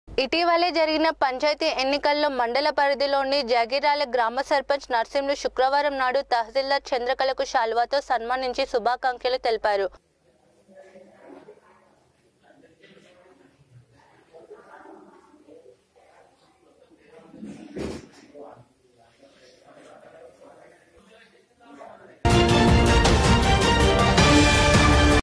జగిర్యాల గ్రామ సర్పంచ్ నర్సింలు తహసిల్దార్ చంద్రకళకు శాలువాతో సన్మానించి శుభాకాంక్షలు తెలిపారు
జగిర్యాల గ్రామ సర్పంచ్ నర్సింలు శుక్రవారం నాడు తహసిల్దార్ చంద్రకళకు శాలువాతో సన్మానించి శుభాకాంక్షలు తెలిపారు